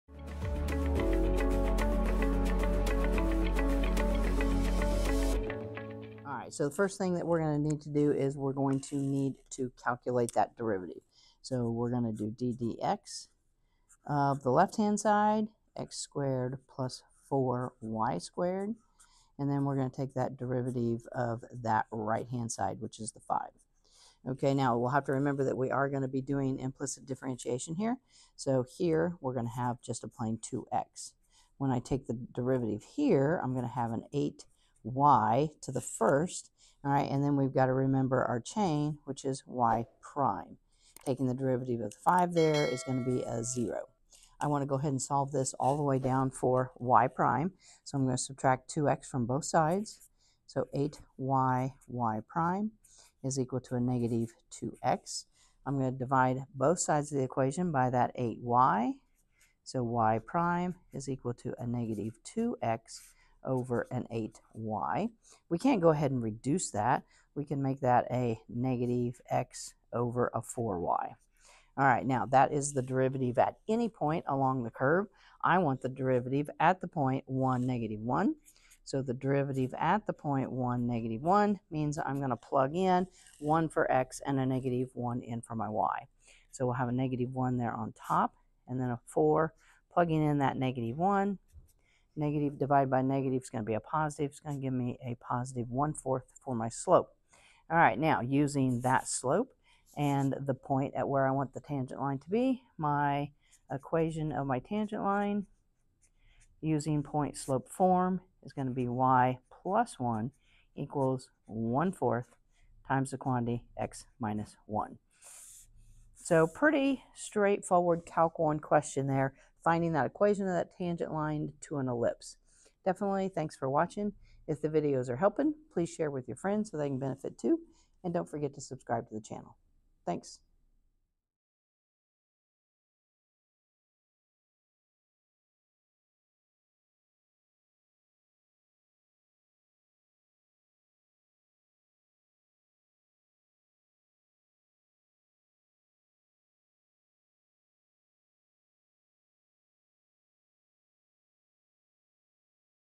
Equation of the Tangent Line to an Ellipse [Calculus]
This video goes through 1 example of writing the equation of the tangent line to an ellipse at a given point.  This is a typical calculus 1 type of question.